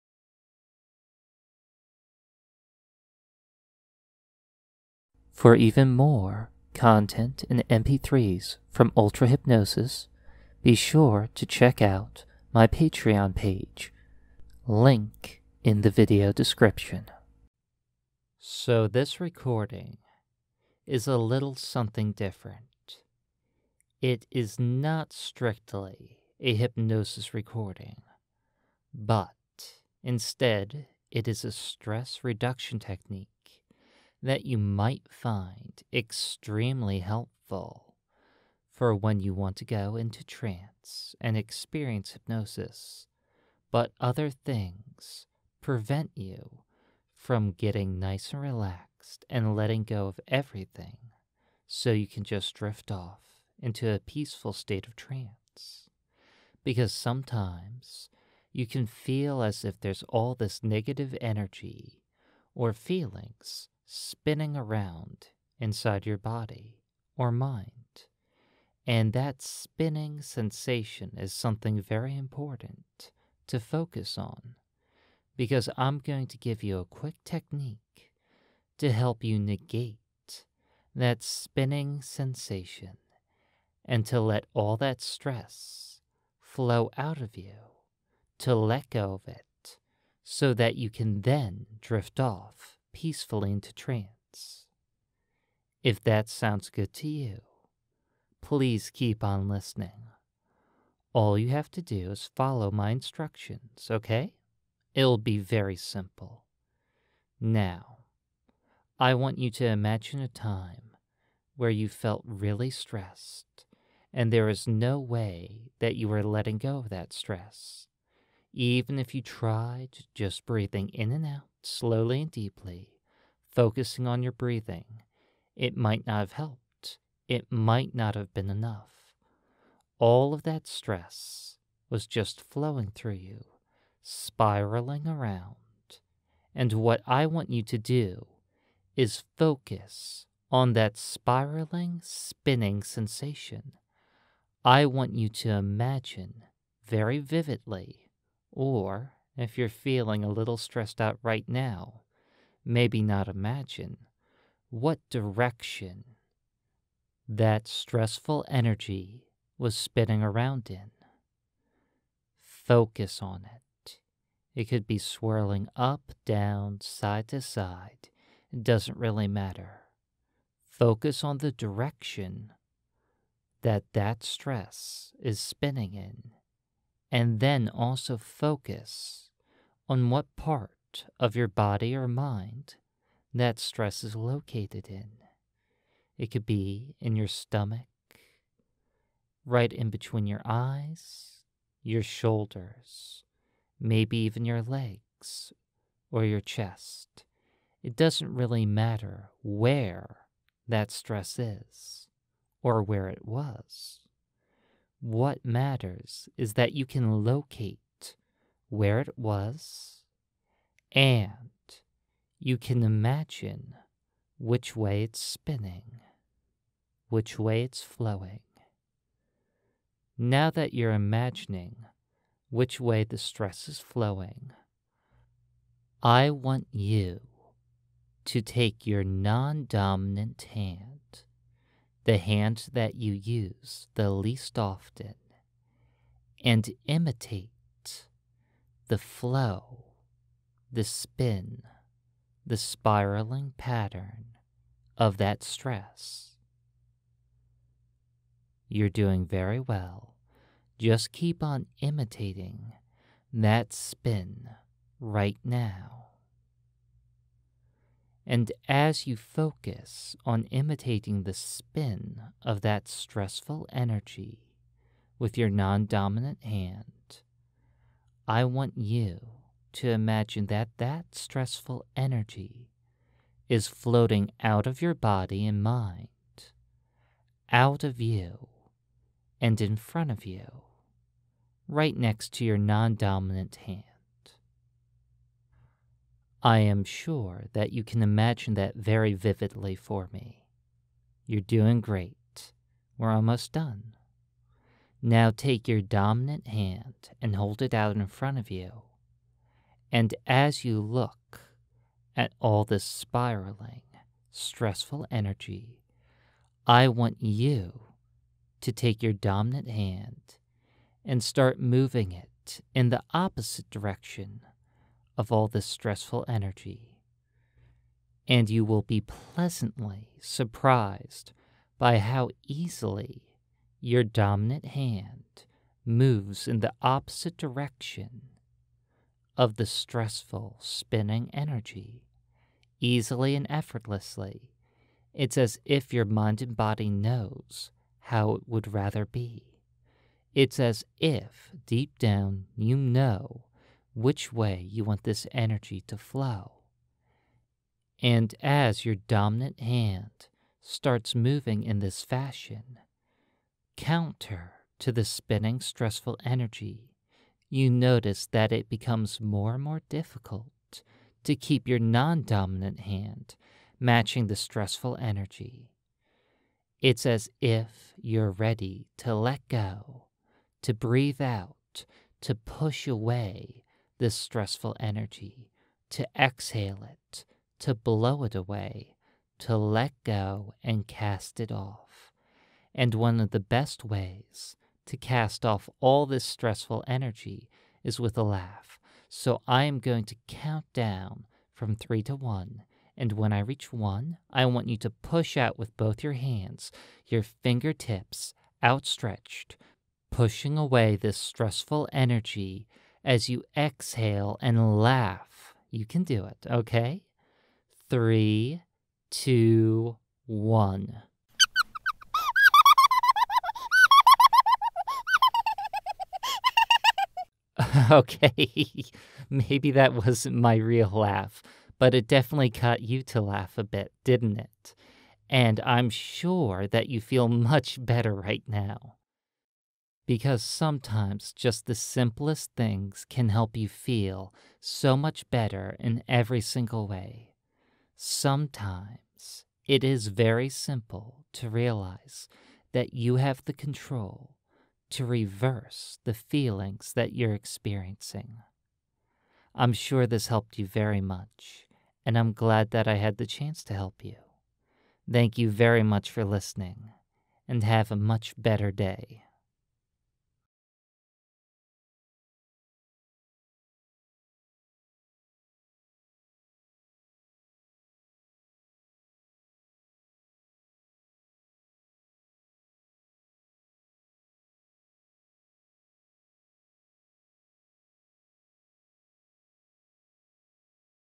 Stress Reduction: Spin Away Stress
This is a free recording that is designed to give to a quick technique that you can use to reduce stress. You can do this technique before you go into hypnosis to make achieving trance easier or you can use it to just help you relax.

This Recording is made possible by the following:
InsaneOtaku
 Seanaxial
Gail Swanson
Scott Waller
LazyboyLP
Koryn Kelly
James Europe
Jayson
SteveFromMass
rayinred
Stephen Atkin
Georgina Giroux
jstsb
steveh
Rick
Blue Violet
gareth
Sam Sara